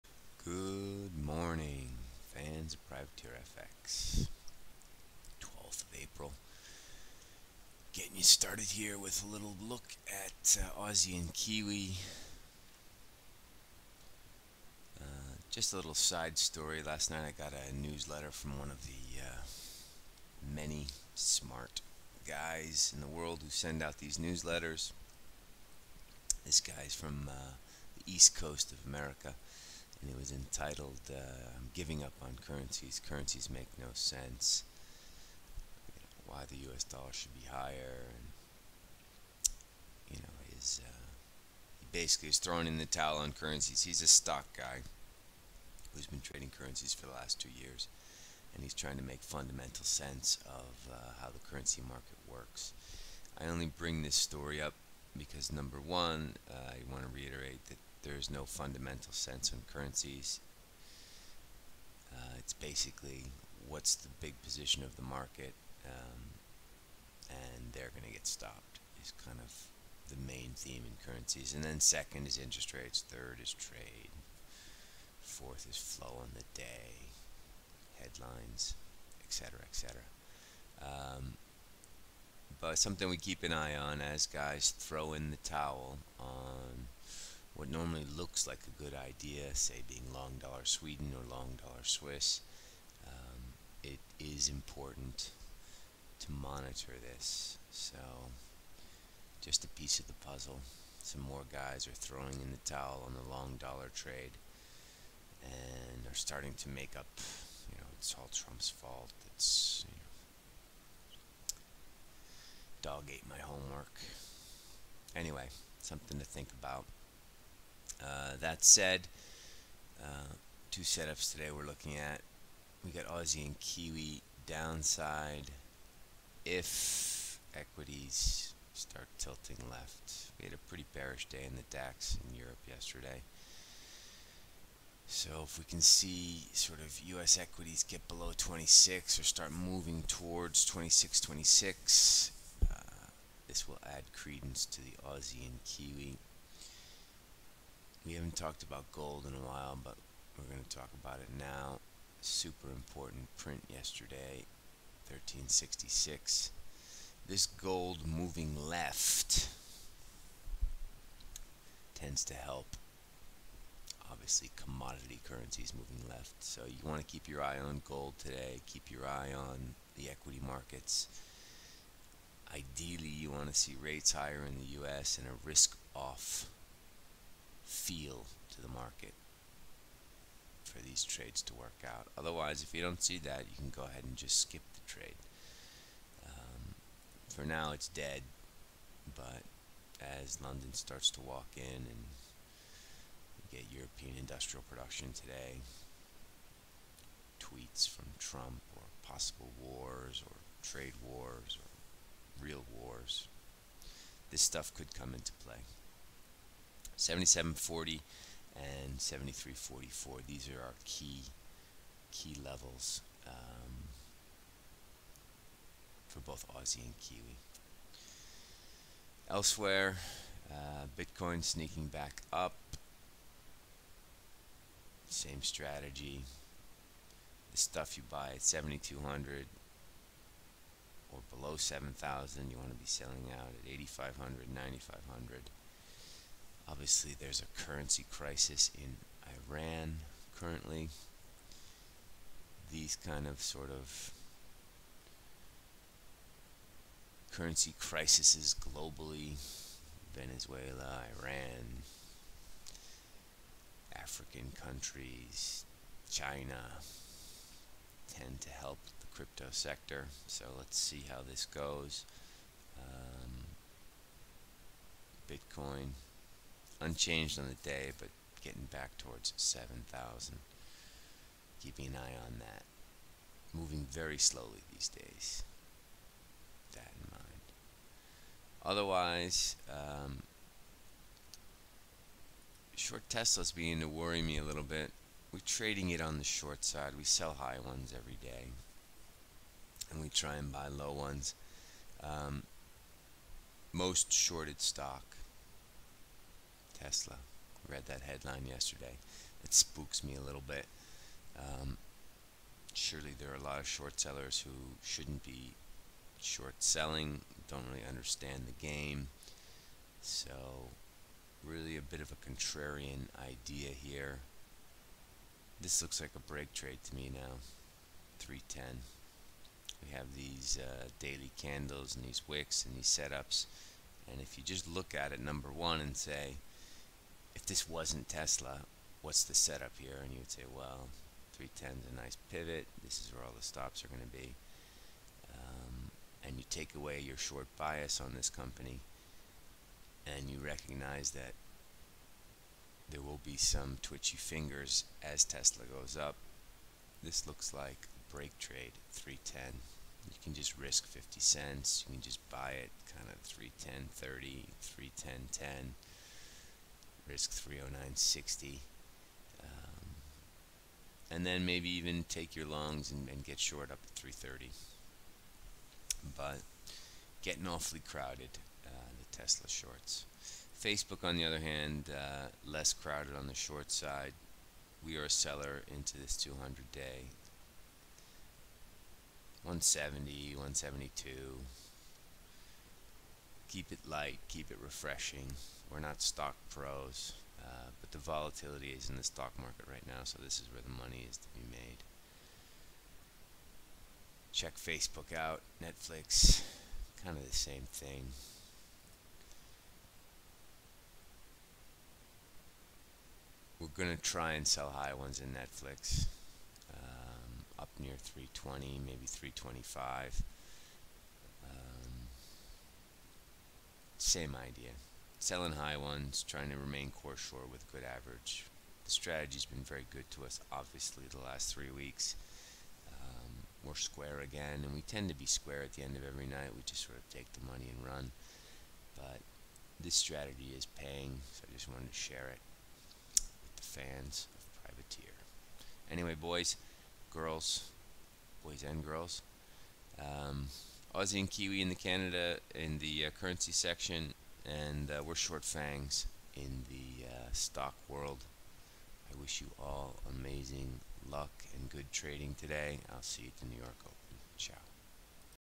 12Apr18 Europe Opening Call - PrivateerFX
Our view on today's European Open the FX & Crypto markets.

No fundamental sense in currencies; 4 themes in FX trading

SetUp; AUDUSD, NZDUSD, Gold/ XAUUSD, TSLA, FB
Crypto; same strategy BTC

Web: PrivateerFX.com

THIS WAS
12Apr18 Europe Opening Call - PrivateerFX